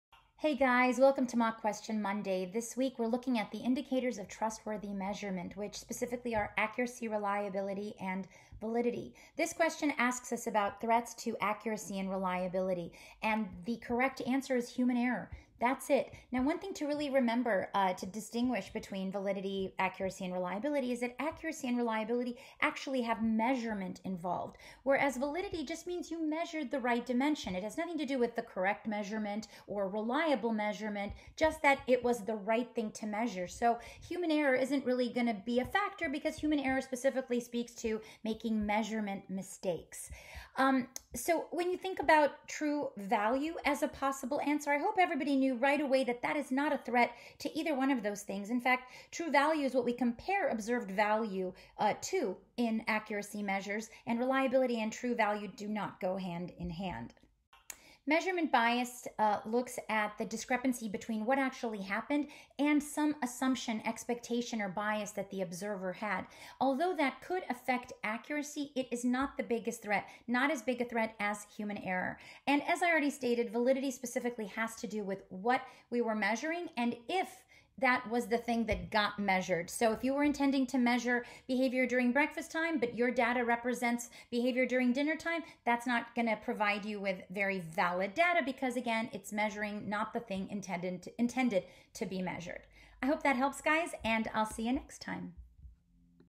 Mock BCBA® Exam Question: The #1 Threat to Accuracy and Reliability (C-8)
Test your measurement knowledge with PTB co-founder Dana Meller as she reviews a mockexam question about the key indicators of trustworthy measurement, revealing the most significant threat facing accuracy and reliability. Dana provides insight into the distinctions between reliability, validity, and accuracy and how they relate to measurement.

Refer to BCBA® Test Content Outline (6th ed.) Domain C.8. Evaluate the validity and reliability of
measurement procedures.

BCBA® Task List (5th ed.) Section C-8: Evaluate the validity and reliability of measurement procedures.